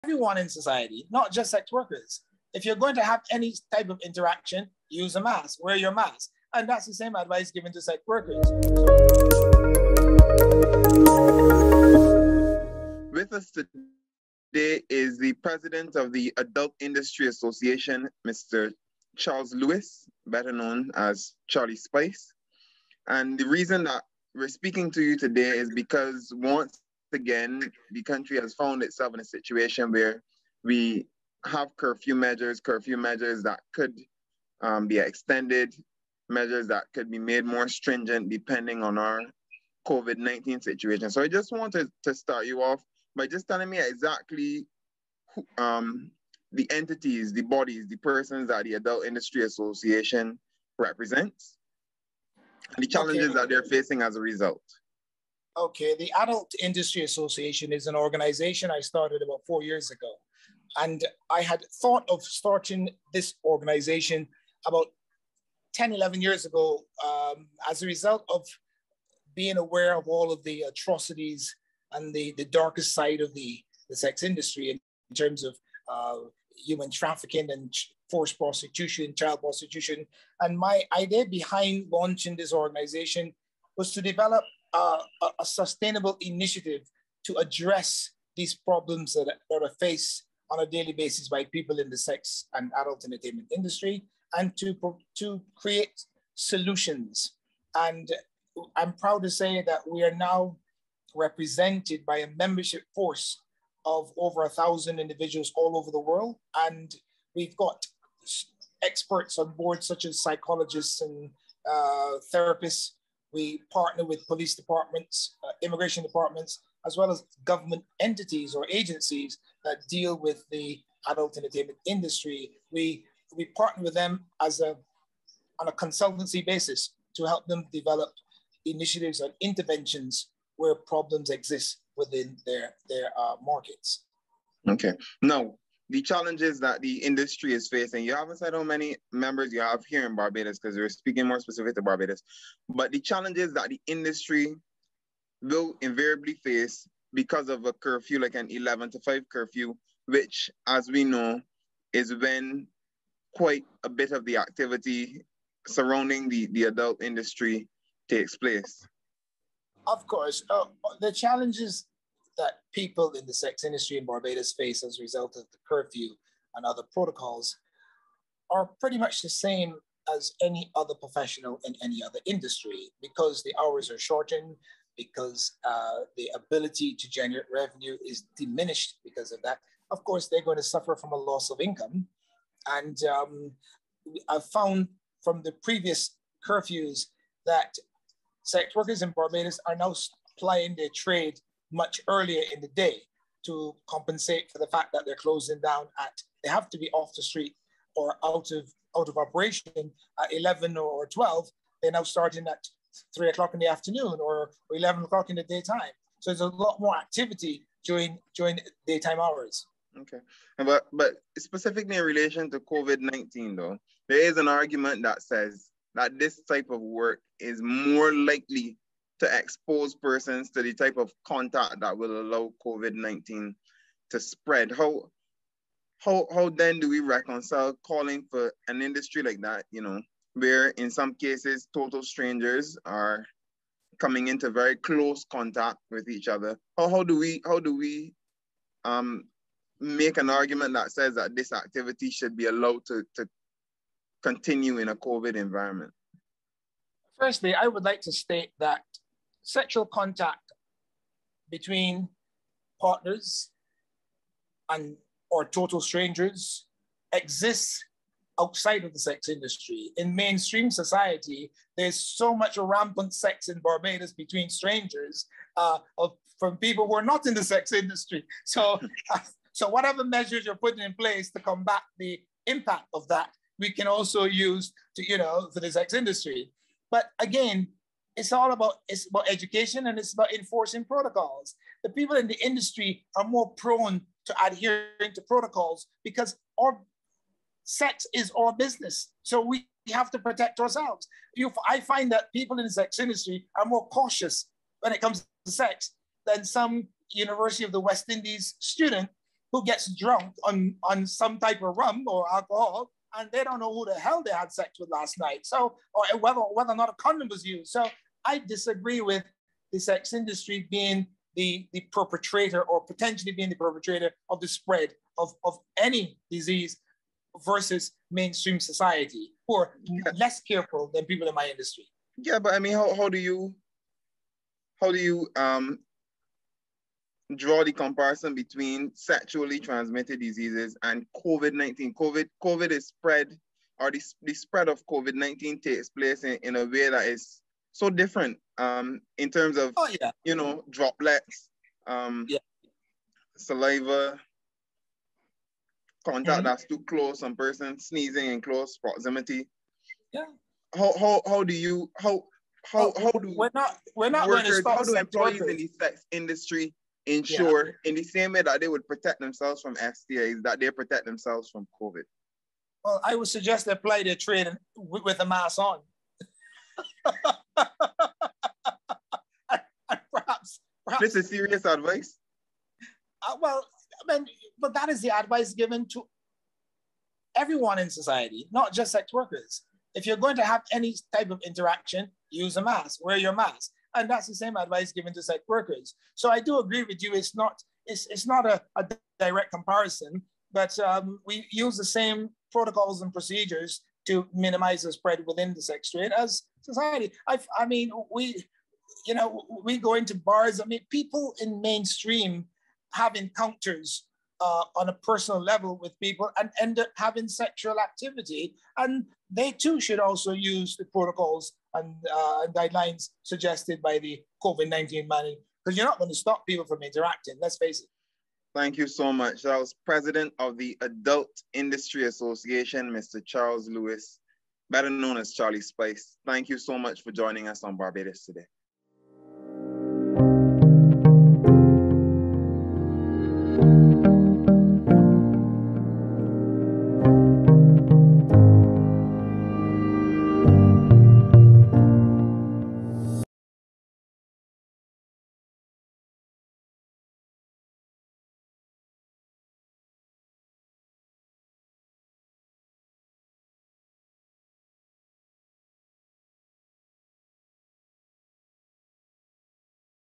Barbados Today News: Lewis says more workers plying trade earlier to beat nighttime shutdowns
The economic fallout from COVID-19 is prompting a “dramatic increase” in the number of people, particularly women seeking careers in sex work, raising questions about the scientific basis for the ongoing night-time curfew.

President of the Adult Industry Association (AIA) Charles Charlie Spice Lewis revealed that with hundreds feeling the pinch of unemployment, they are approaching him to discuss alternative options to make ends meet and are finding more creative ways to do so.